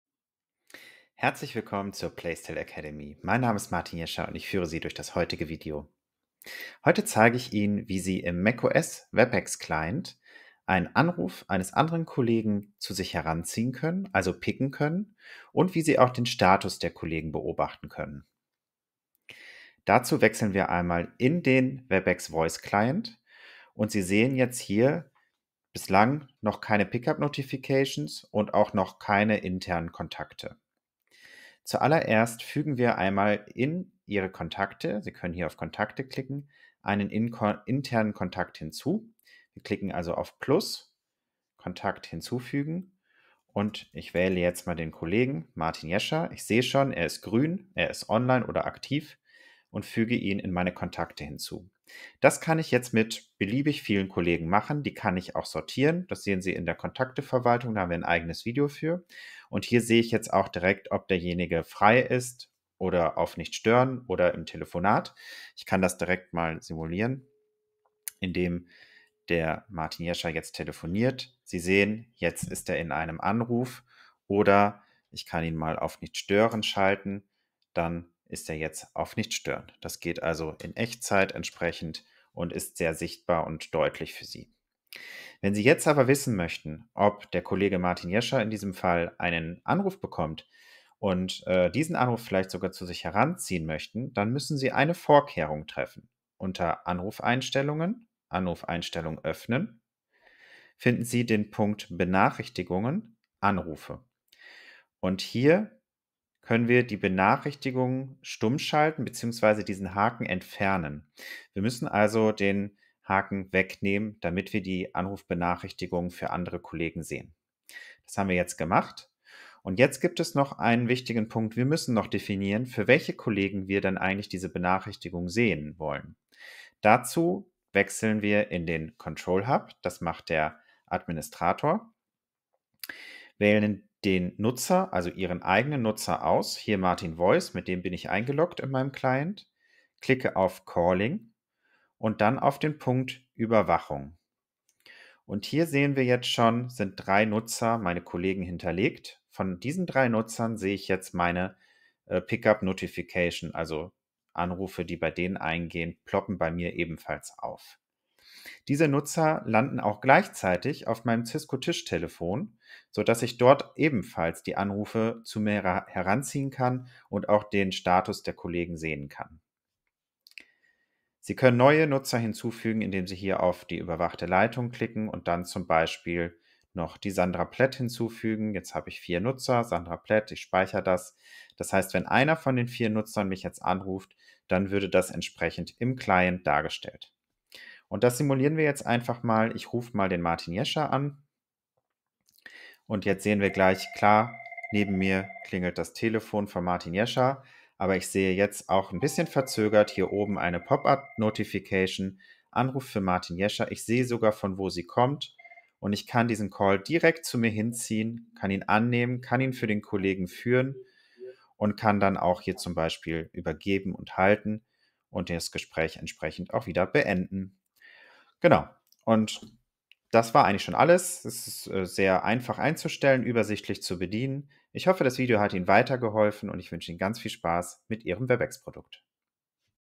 BLF und Pickup im Webex Client
In diesem Video erklären wir Ihnen, wie Sie die BLF-Funktion und die Pickup-Funktion im Webex Client für macOS einrichten. Mit Besetztlampenfeldern können Sie den Status Ihrer Arbeitskollegen sehen und via Pickup die Anrufe Ihrer Kollegen zu sich heranziehen und sie entgegennehmen.